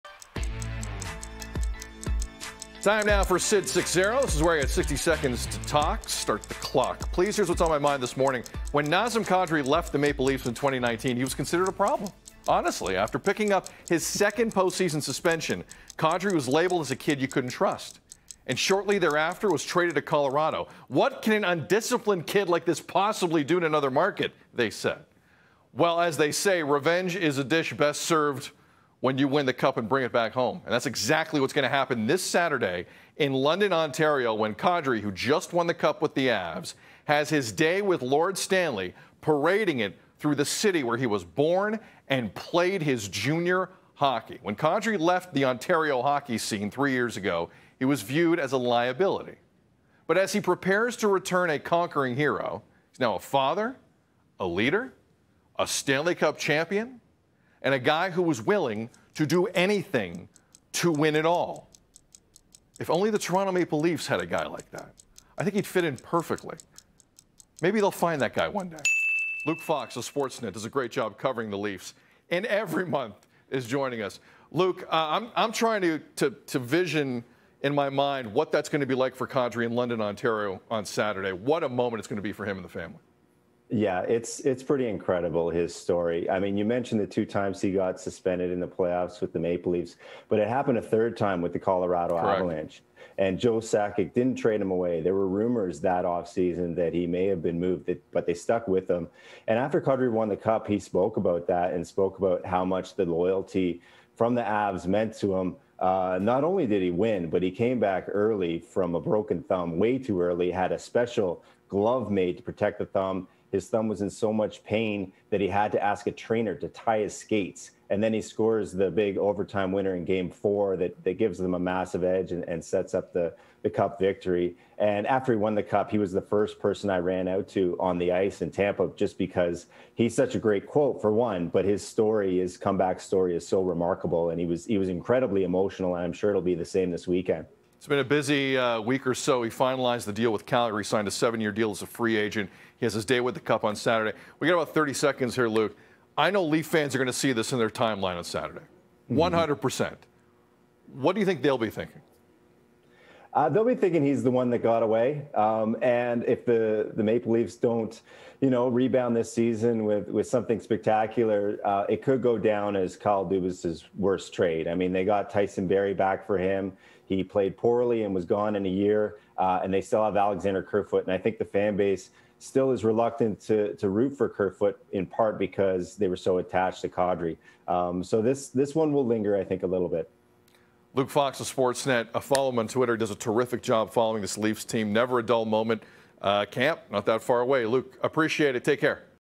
Nazem Kadri is bringing the Stanley Cup home
Sid is joined by Sportsnet’s Luke Fox to discuss Nazem Kadri’s experience in the NHL thus far, and how this weekend he will be bringing the Stanley Cup to visit his hometown.